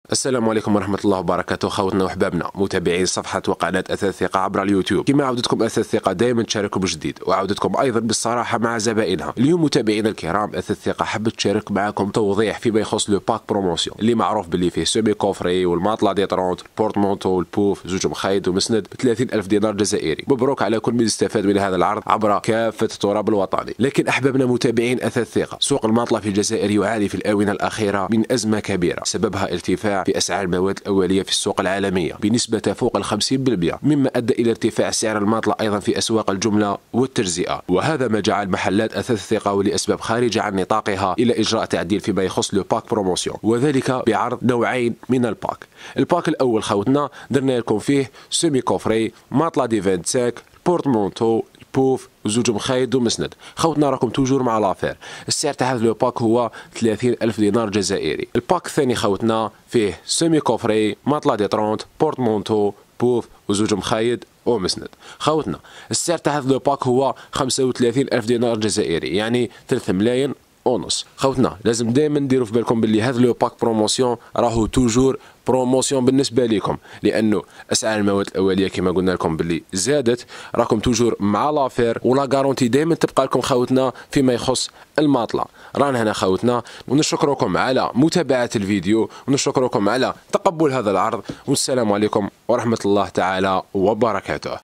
شاهد الباك بروموسيون الجديد مع الأسعار الذي فيه كل لوازم غرفة النوم عند أثاث الثقة بالقليعة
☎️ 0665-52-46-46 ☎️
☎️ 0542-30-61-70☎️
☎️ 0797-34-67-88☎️